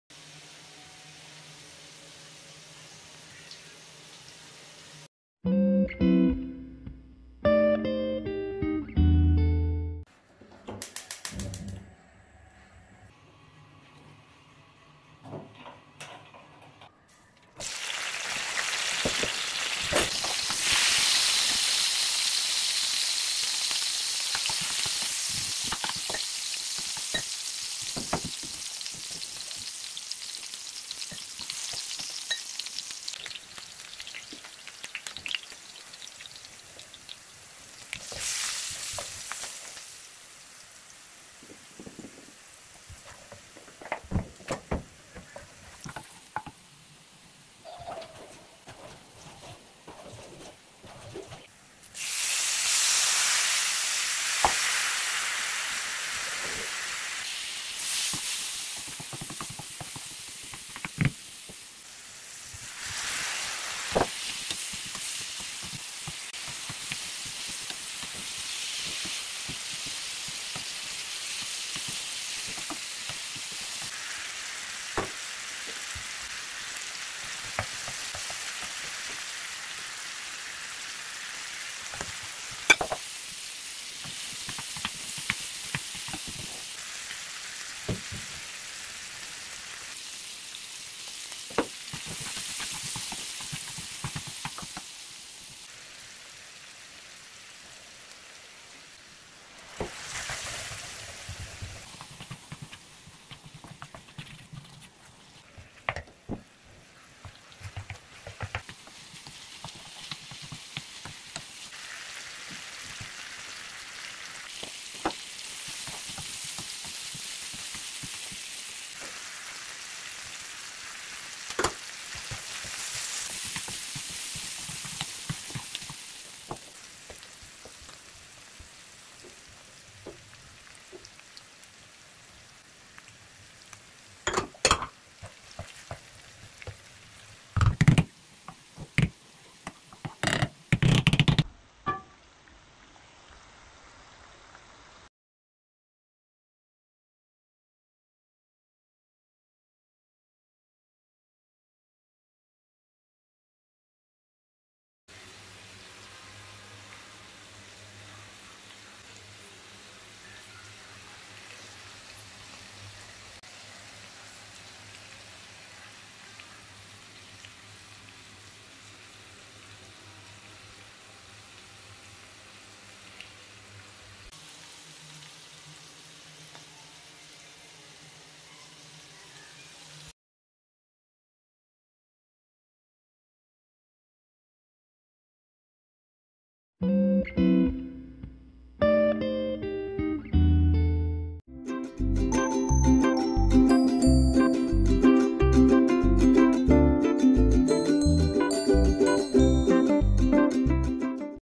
Spicy Fried Tumeric Beef - Resepi Daging Goreng Kunyit Pedas
Resepi kali ini ialah daging goreng kunyit yang mudah,cepat  dan sedap, makan asik nak tambah aje...yg penting GULA & GARAM kene cukup rasa ye..baru kick..klu rasa kurang tu, tambah aje dua benda ni betul2 sukatan,xkan xsedap punye lauk yang kita masak... 😁